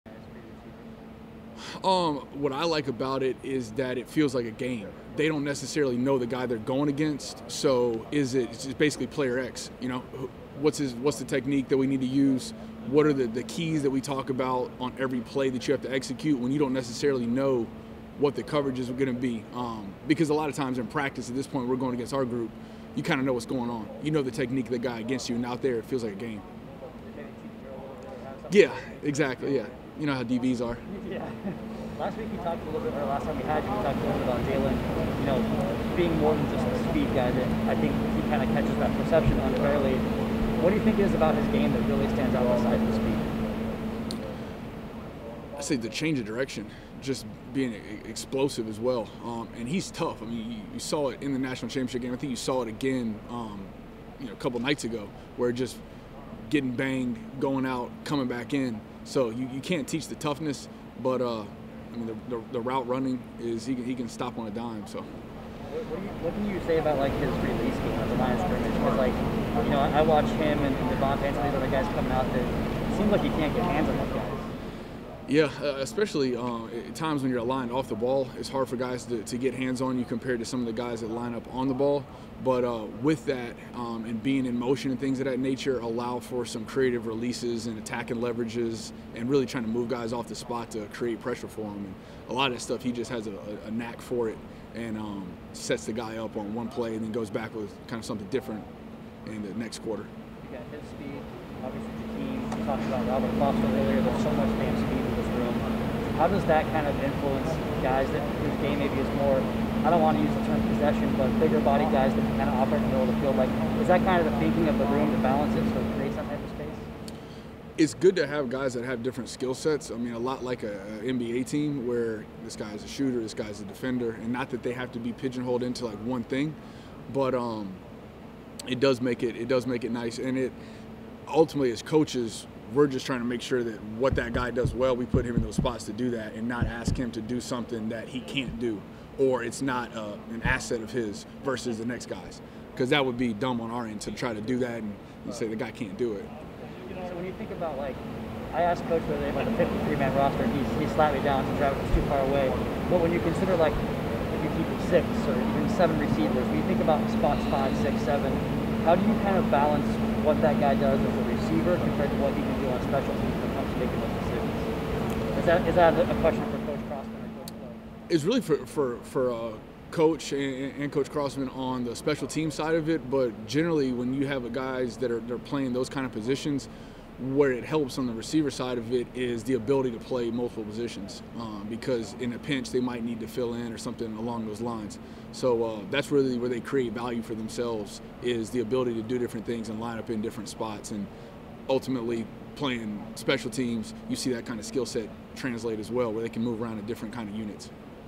Coach Grizzard discusses Training Camp | Miami Dolphins
WR Coach Josh Grizzard met with the media on August 23 to discuss the continuation of Training Camp, presented by Baptist Health.